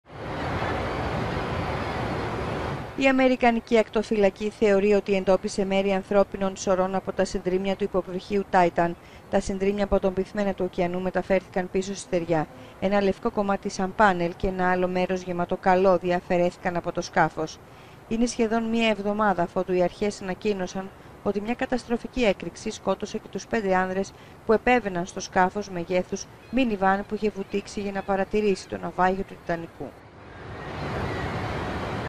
Συνεχίζονται οι έρευνες γύρω από την τραγωδία με το μίνι υποβρύχιο Titan
Τι ανακάλυψε η ακτοφυλακή στον πυθμένα του βυθού